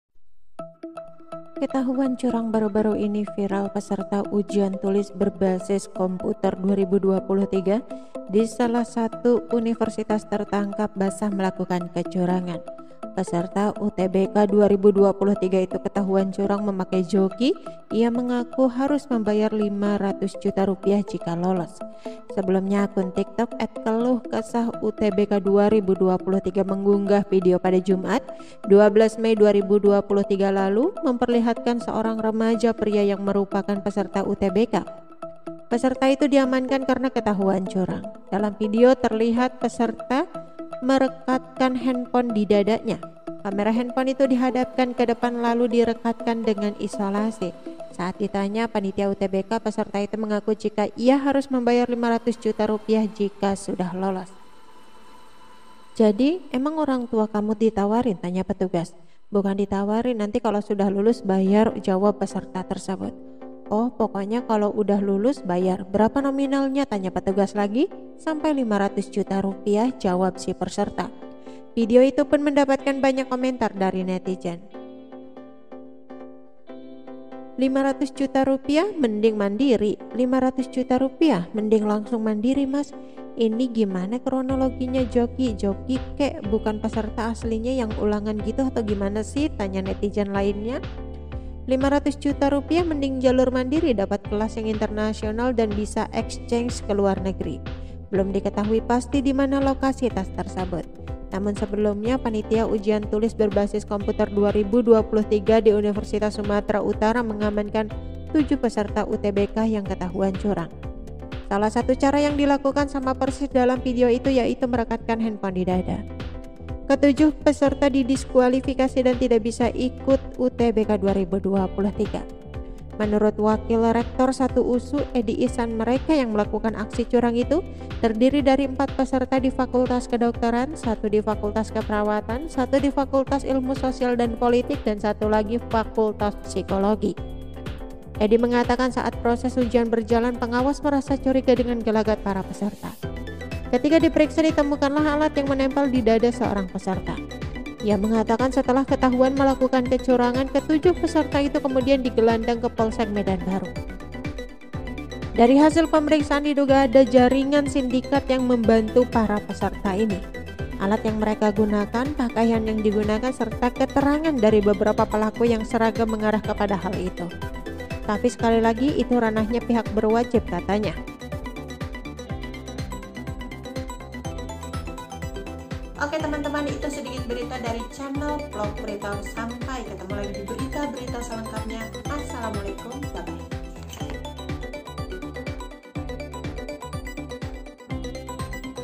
KETAHUAN Curang, Peserta UTBK 2023 Tempel HP di Dada, Bayar Rp 500 Juta Jika Lolos, Nasibnya Kini
Ketahuan curang! Baru-baru ini viral peserta Ujian Tulis Berbasis Komputer (UTBK) 2023 di salah satu universitas tertangkap basah melakukan kecurangan.

Peserta UTBK 2023 itu ketahuan curang memakai joki, ia mengaku harus membayar Rp 500 juta jika lolos.

Sebelumnya, akun TikTok @ keluhkesahutbk23 mengunggah video pada Jumat (12/5/2023) lalu memperlihatkan seorang remaja pria yang merupakan peserta UTBK.